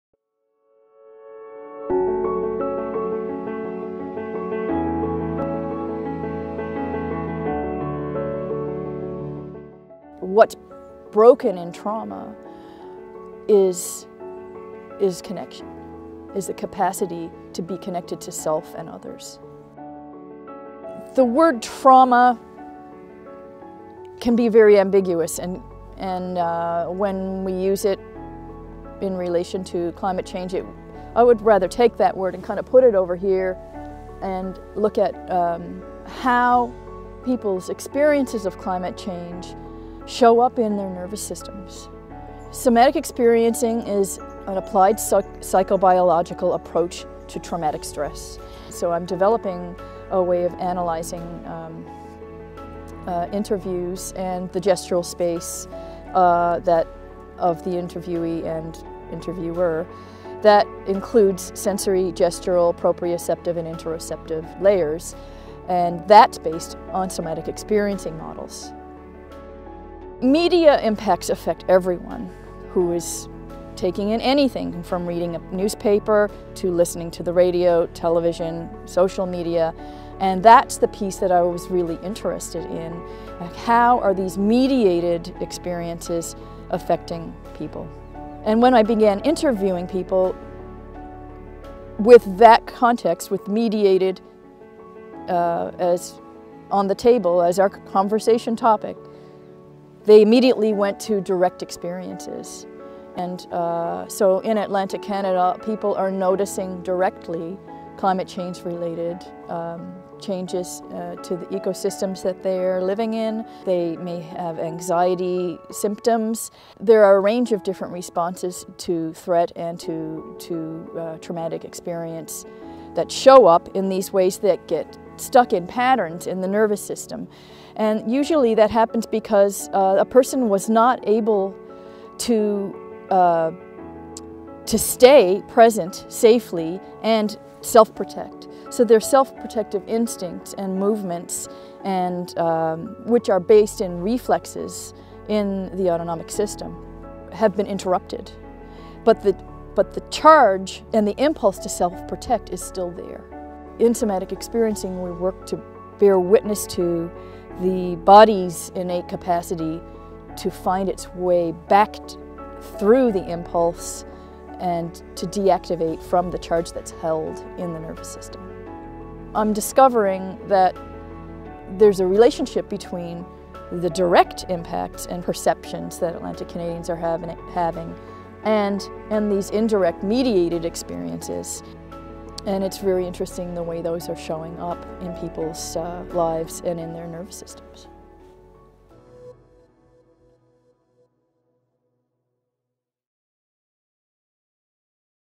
Lori Clarke | What's your master plan?
Lori Clarke says an interdisciplinary PhD gave her the opportunity to explore a real world problem from a new perspective.